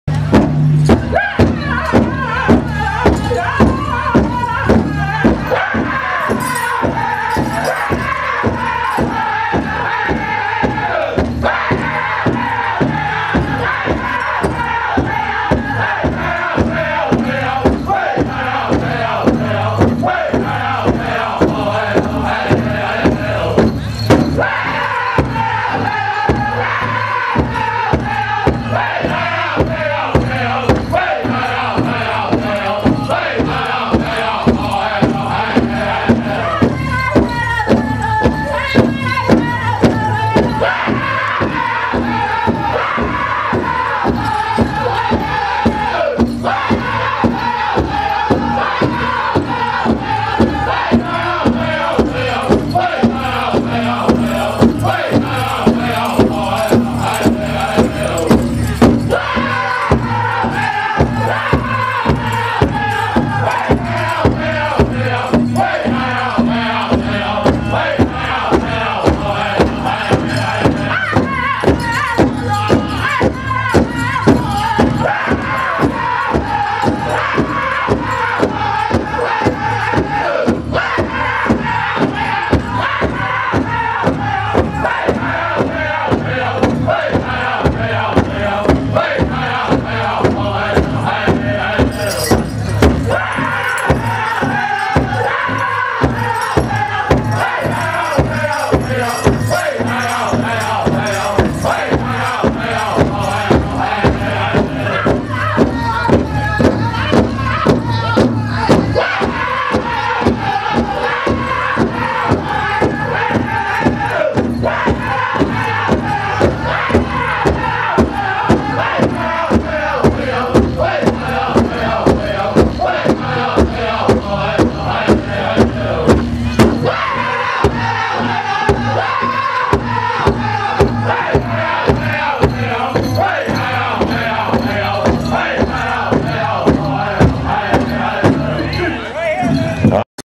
Womens Fancy Dance in Obashing September 2011
Obashing Labor Day Powwow in Ponemah September 2-5, 2011